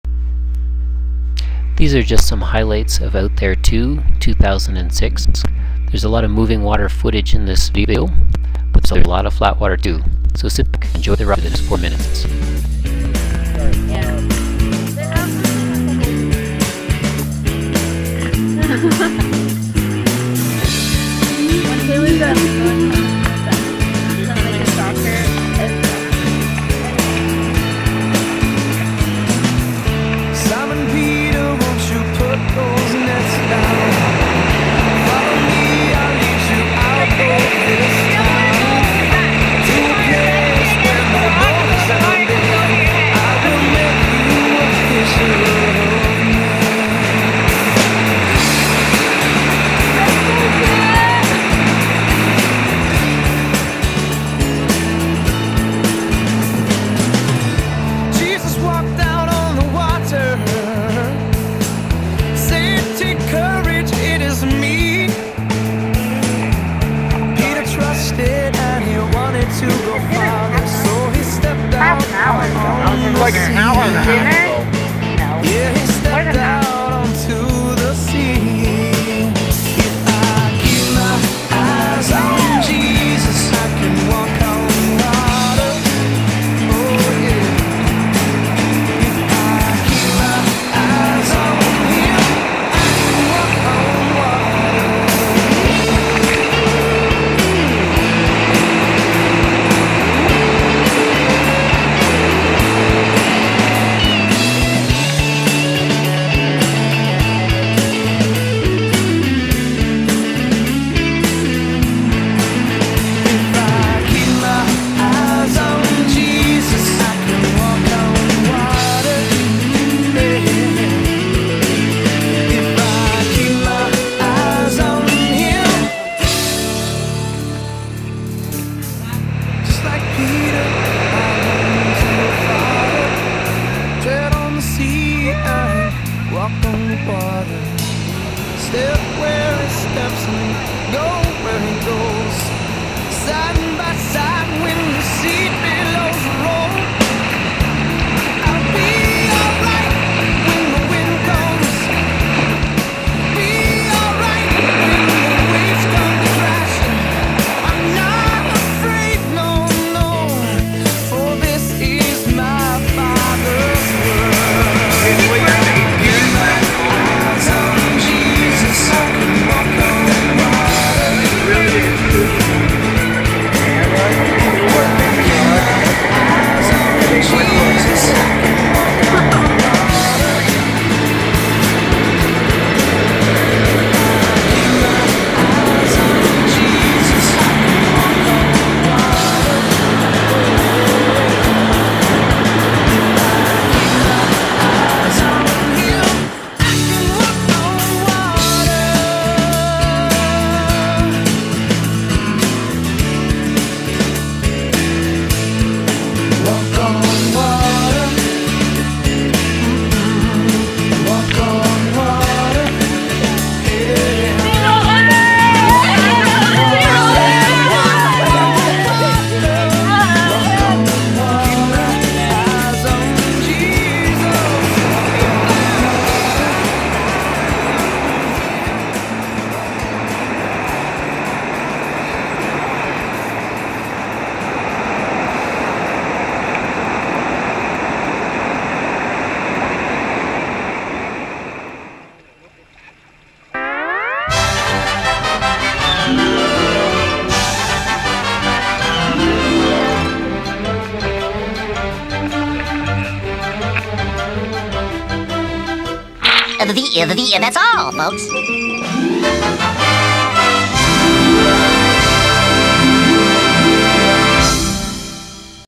Camp IAWAH Venture Noire River
This is a sample of our Noire River trip in western Quebec for our Venture program.